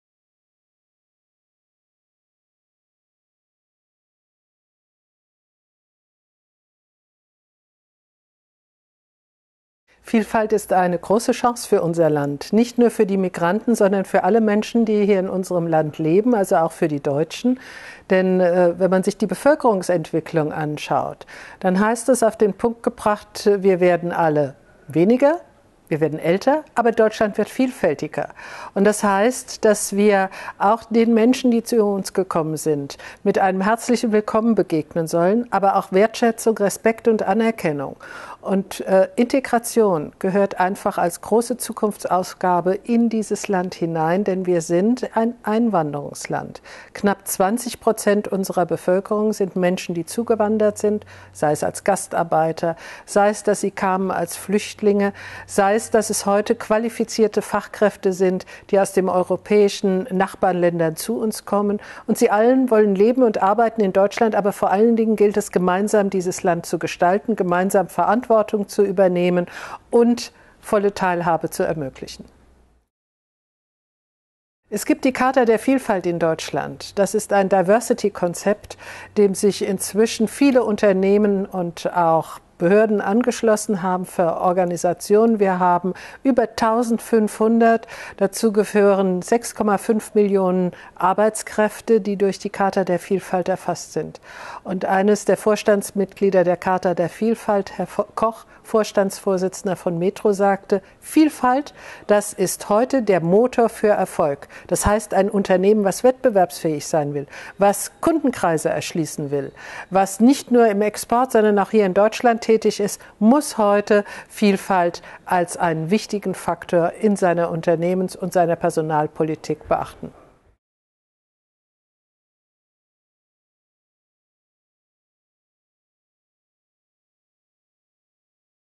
Prof. Dr. Maria Böhmer im KAS-Interview
Die Integrationsbeauftragte des Bundes Prof. Dr. Maria Böhmer über kulturelle Vielfalt